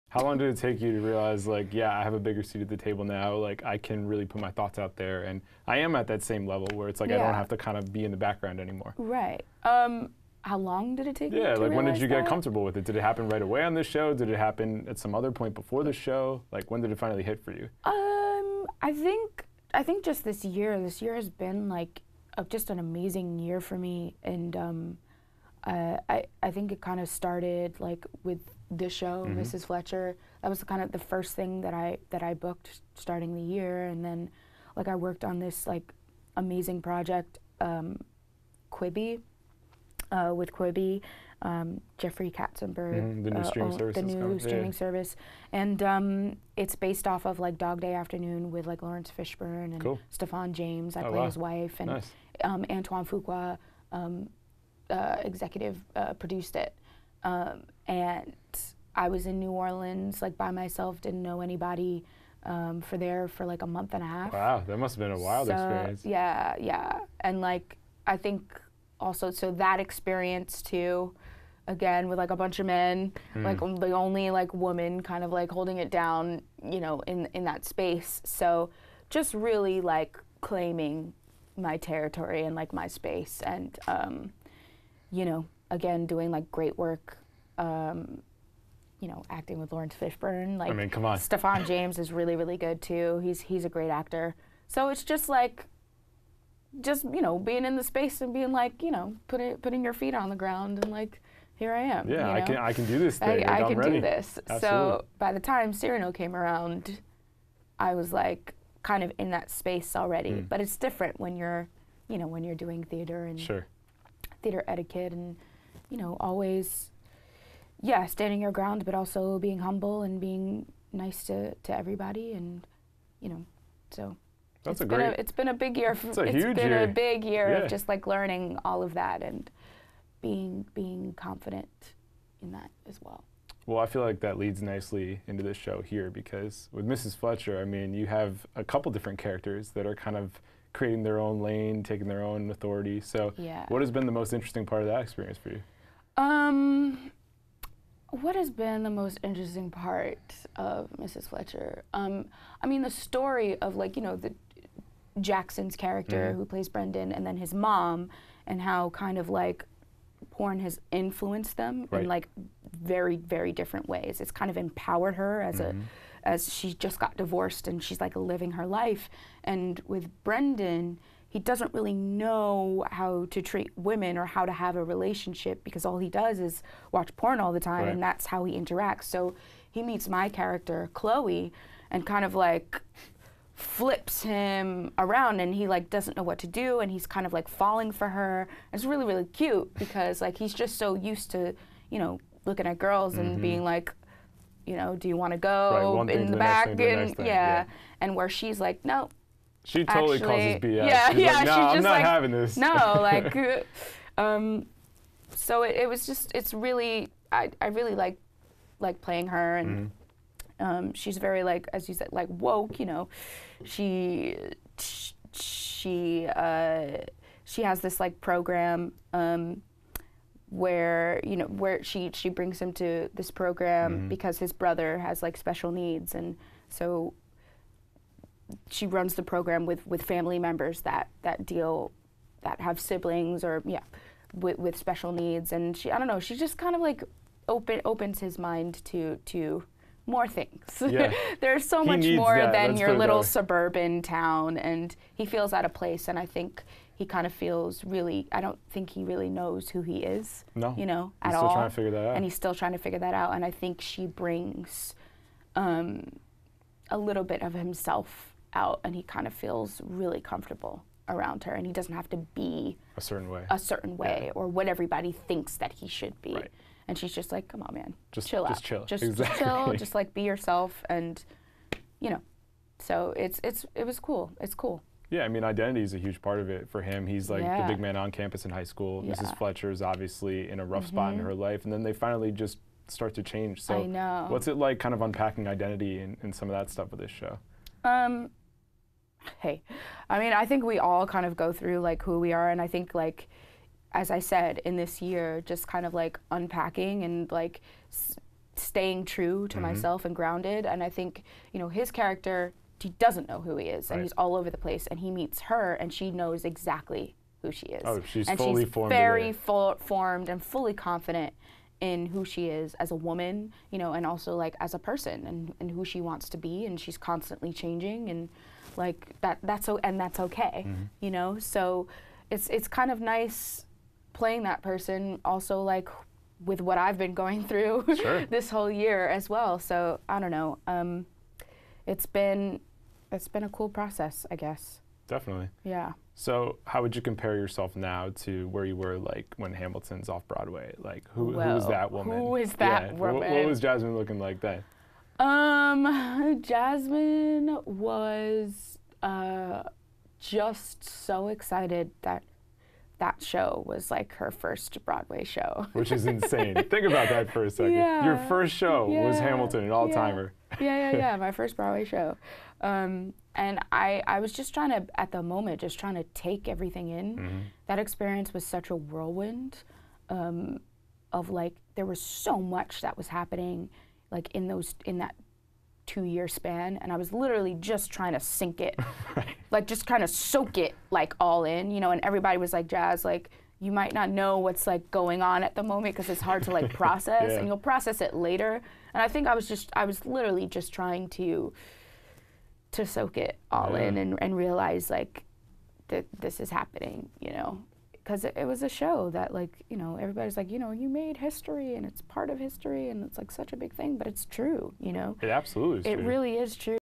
Jasmine Cephas Jones On "Hamilton," "Mrs. Fletcher," "Cyrano"
Cephas Jones talks with DJ Sixsmith about her magical run on Hamilton, her new play with Peter Dinklage and working with Kathryn Hahn.